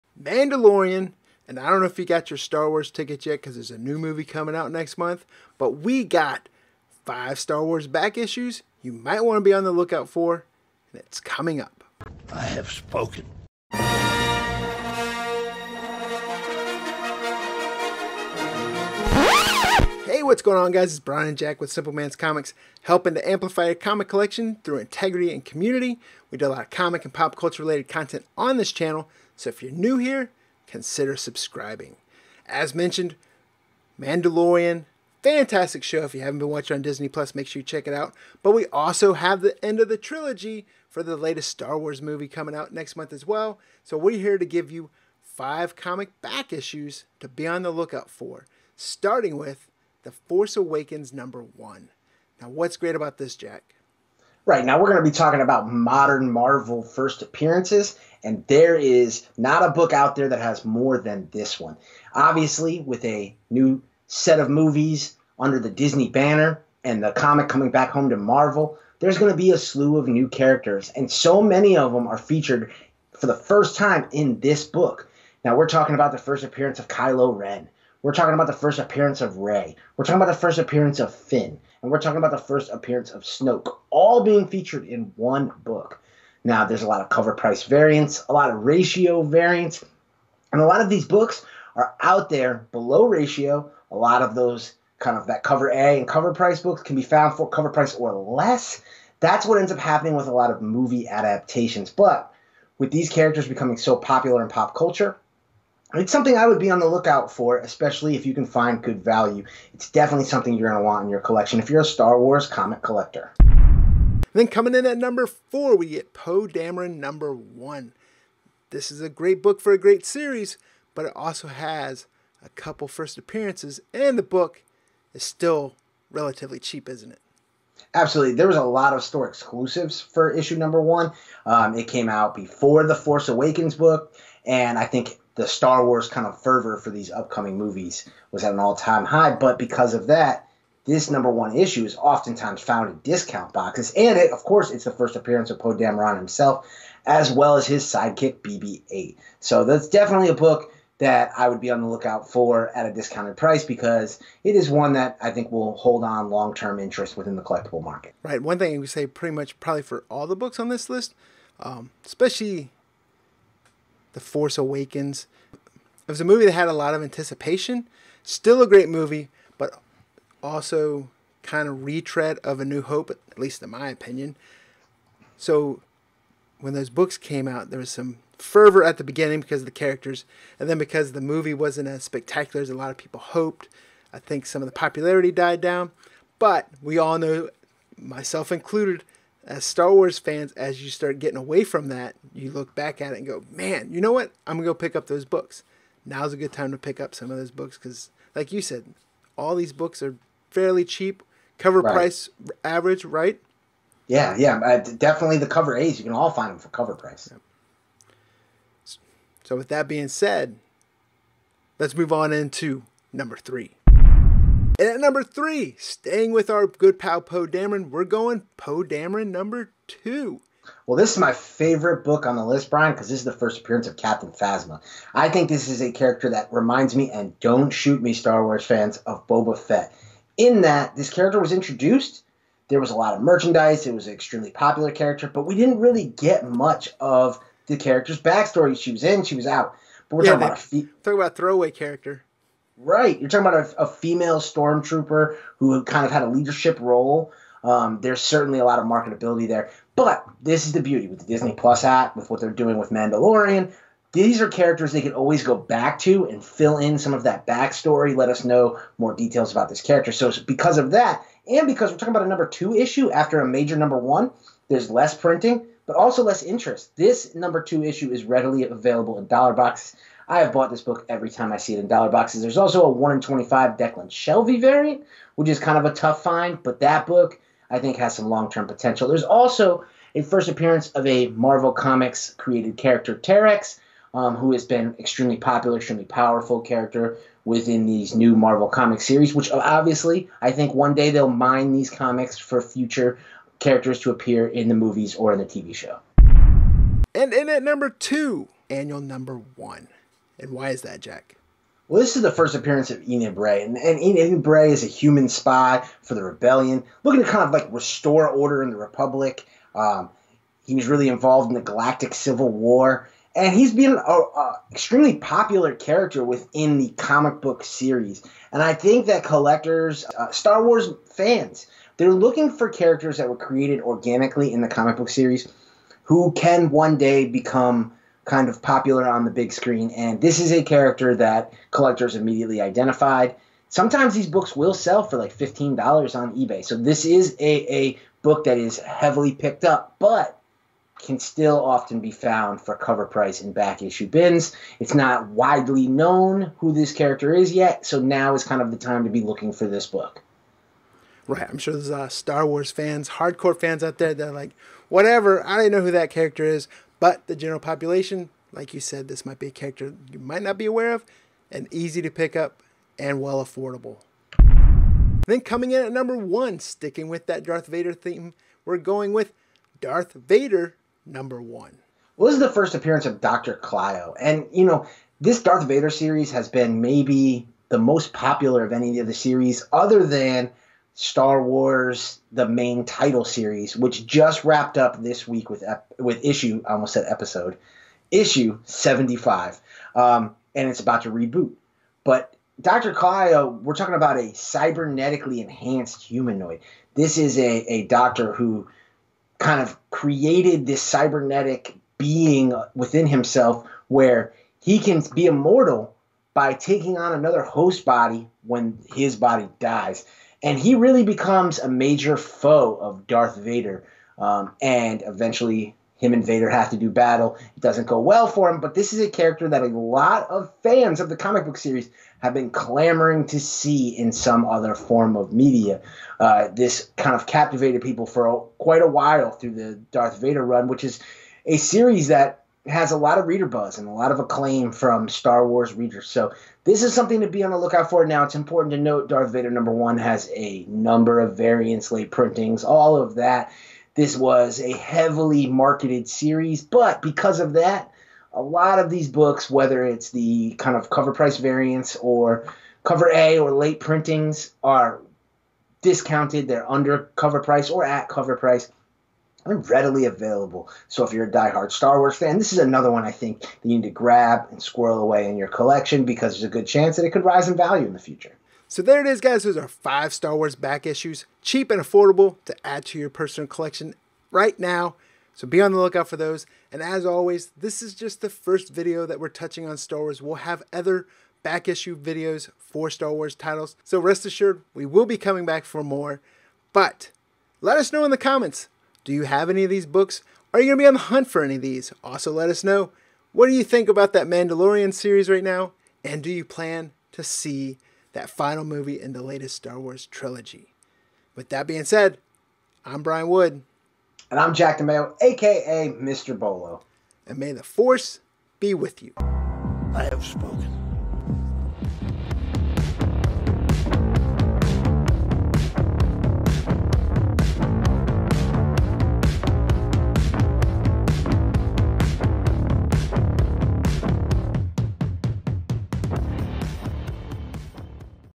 5 Modern Marvel Star Wars Comics to Be On the Lookout For Pt I
We discuss 5 modern Marvel Star Wars comic book back issues that are very affordable and worth adding to your comic book collection. Full of first appearances and key issues.